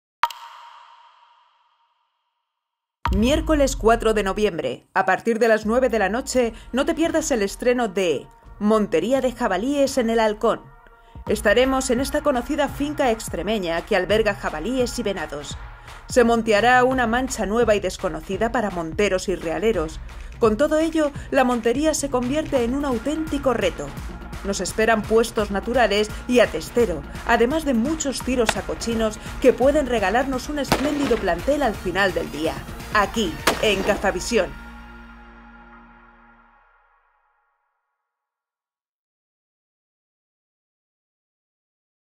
Montería de jabalíes en el Halcón
Estaremos en esta conocida finca extremeña que alberga jabalíes y venados. Se monteará una mancha nueva y desconocida para monteros y rehaleros. Con todo ello, la montería se convierte en un auténtico reto. Nos esperan puestos naturales y a testero, además de muchos tiros a cochinos que pueden regalarnos un espléndido plantel al final del día.